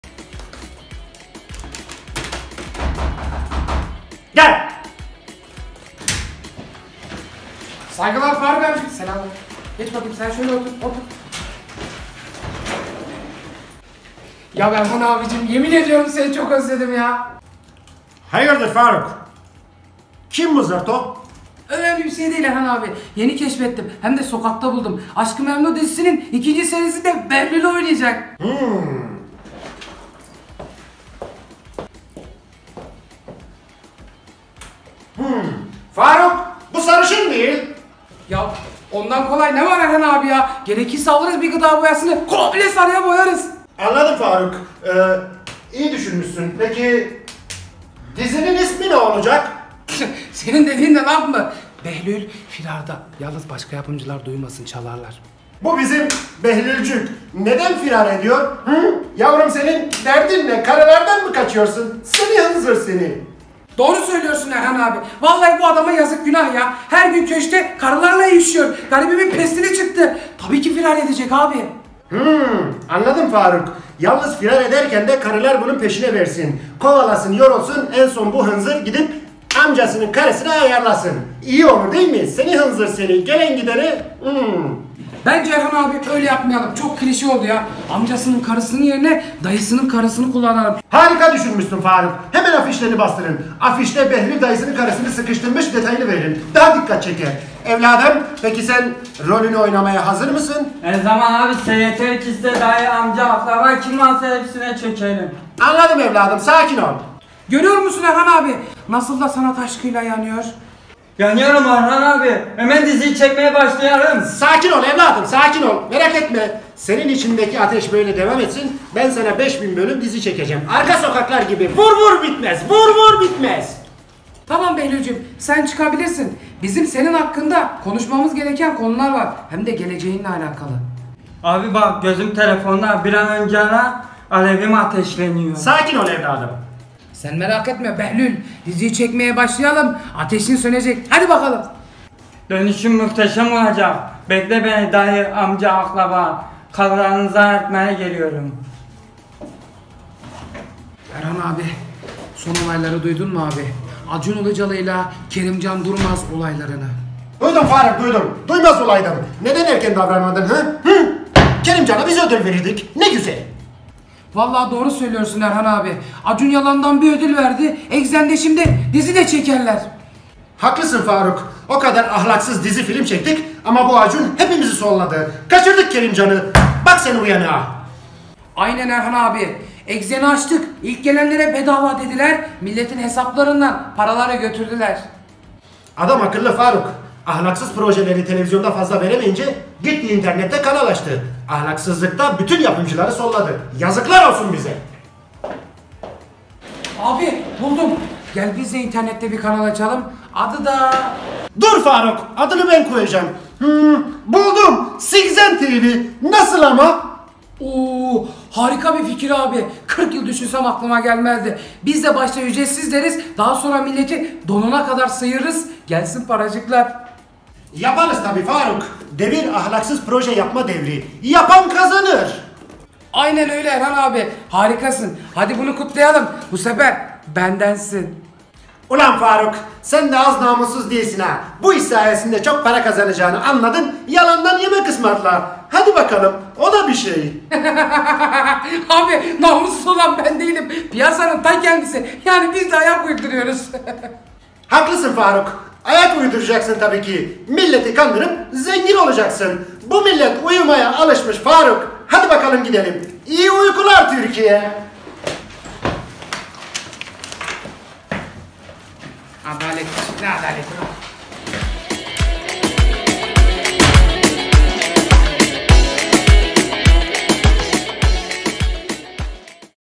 ÜÇ KAĞITÇI YAPIMCI 4.BÖLÜM
Ahlaksız yapımcıları konu aldık.. televizyon dünyası tamamen insanları uyutmak için çalışmaktadır.. dikkat..!! iyi seyirler

Oyuncular

Cüneyt Özen
Burak Doğan
Ersin Burgaç